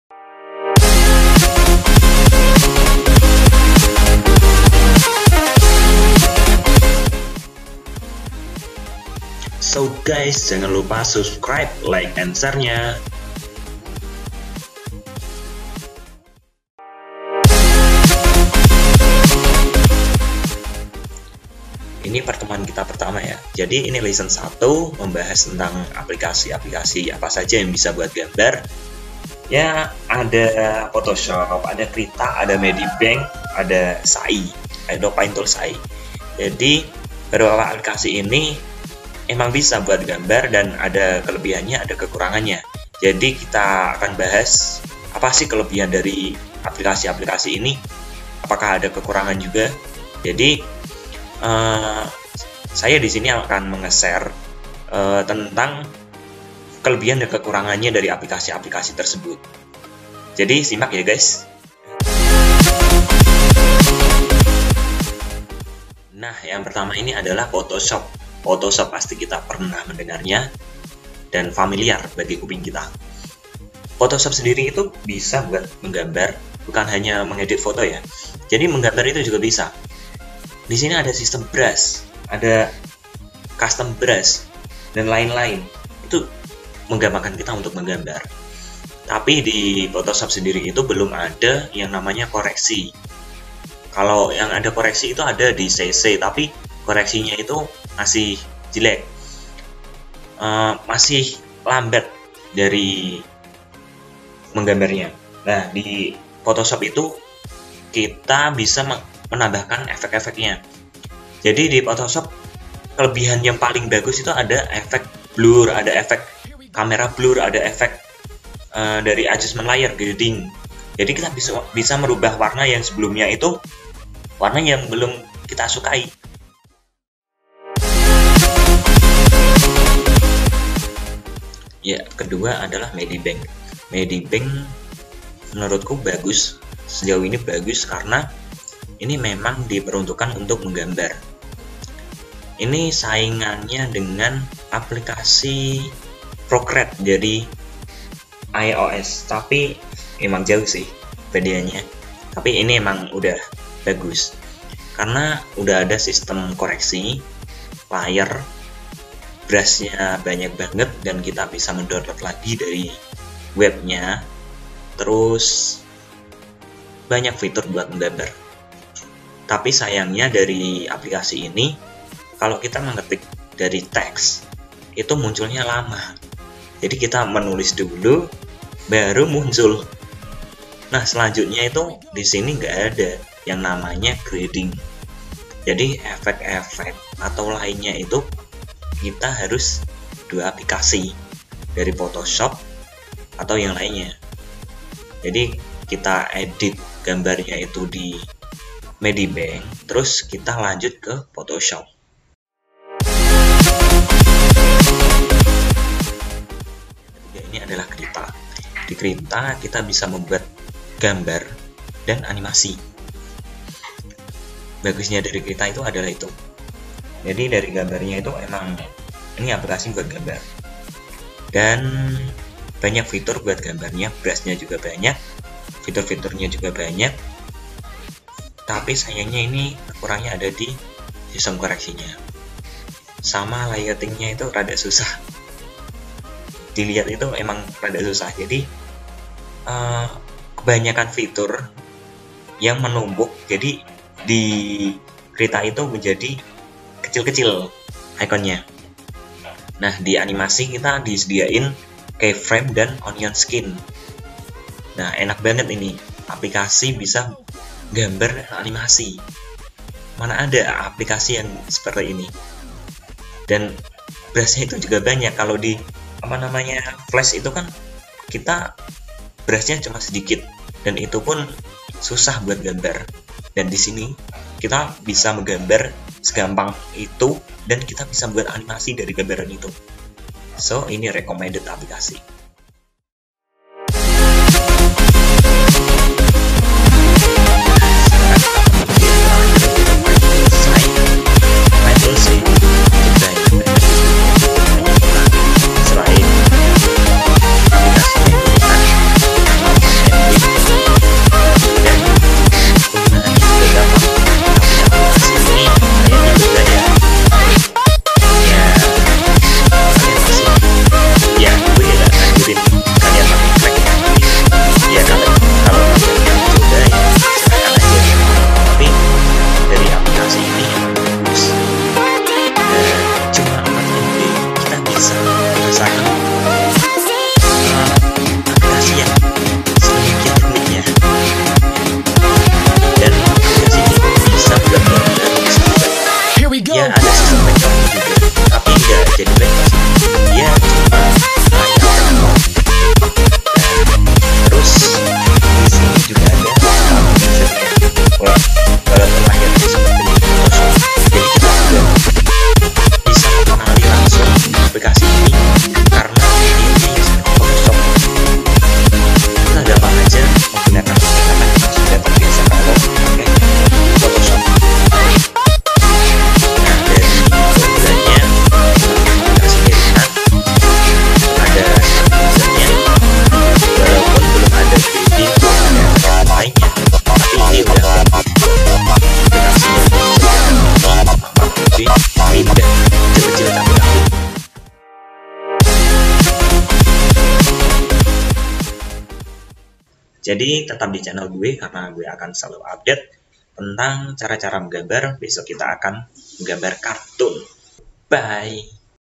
[APLIKASI GAMBAR KARTUN] apa aja sih?
Jadi kalian yg ingin menggambar di komputer, gua punya saran nih aplikasi2 nya, apa aja sih? Yuk kita simak.